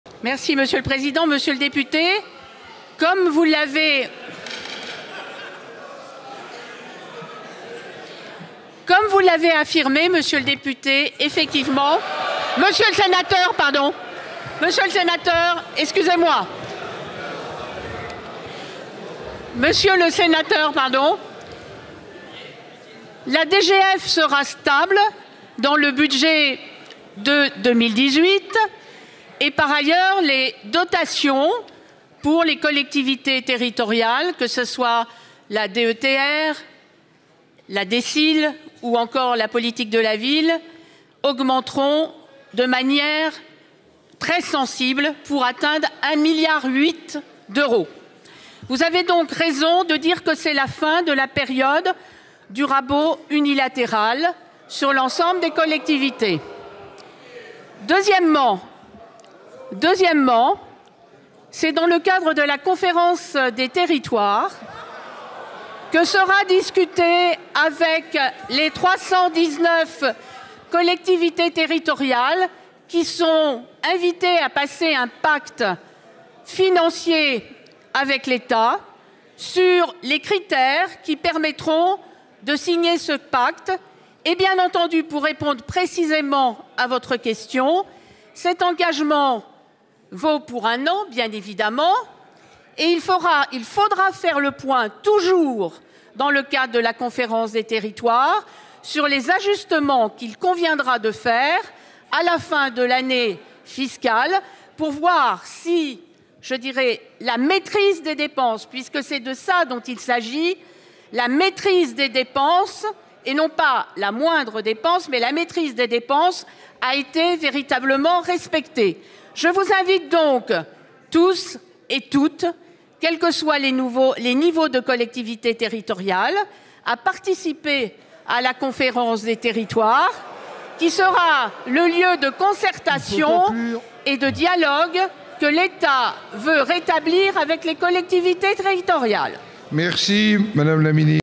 Questions d’actualité : Gourault chahutée sur les dotations aux collectivités
Séance animée au Sénat pour la reprise des questions au gouvernement. Pour sa première question, le sénateur (La République en marche) de Paris Julien Bargeton a choisi d’interpeller Jacqueline Gourault, ministre auprès du ministre de l’Intérieur Gérard Collomb, et spécialisée sur les collectivités locales.

Certaines annonces gouvernementales « ont suscité des interrogations, parfois compréhensibles, et parfois un peu caricaturales », a-t-il exposé. « Le gouvernement a voulu jouer cartes sur table lors de la Conférence nationale des territoires. » Clameur dans l’hémicycle.

L’agitation reprend de plus belle pendant sa question. « Au moment où ce projet de loi de finances met enfin un terme aux baisses de dotations aux collectivités territoriales – ce qui est inédit – pouvez-vous nous assurer que cette stabilité durera pendant tout le quinquennat ? » [...]